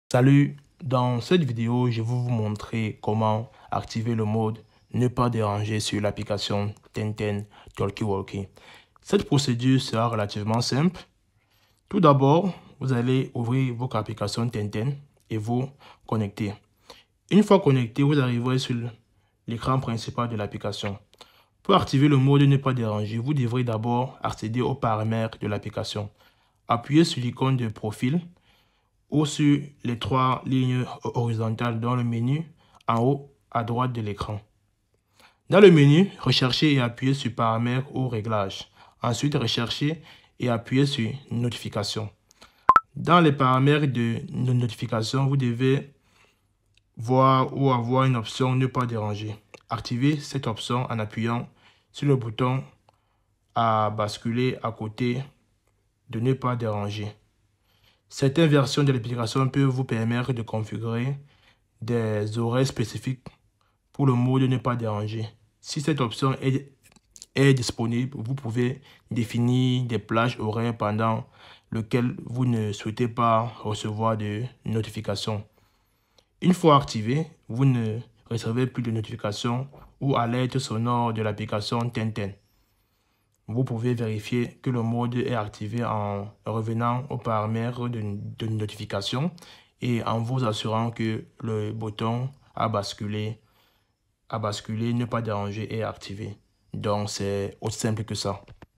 Comment Mettre Ne Pas Deranger Sur Ten Ten (2025)
Dans cette vidéo, vous apprendrez exactement Comment Mettre Ne Pas Deranger Sur Ten Ten